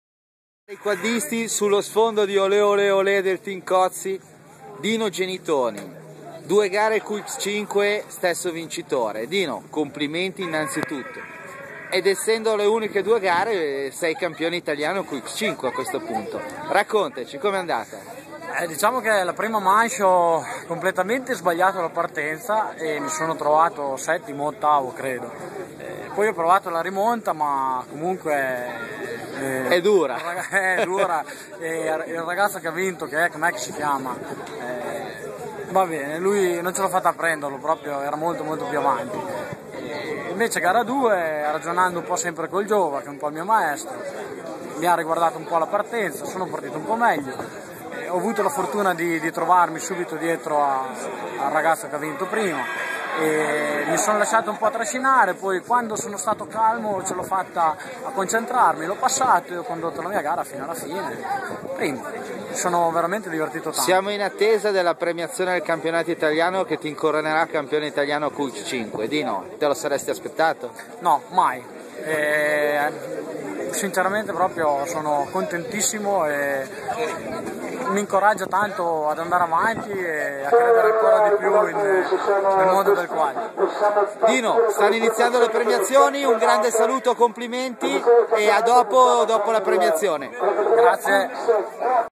videointervista a Dino Genitoni, team yamaha Errezeta, Campione Italiano Quad Cross 2011 QX5, diritti di Quaddisti.it, videoarticolo completo pubblicato su Radio Box di Quaddisti.it , 2011_10_09 settima prova campionato italiano quad cross fmi 2011 e premiazioni finalli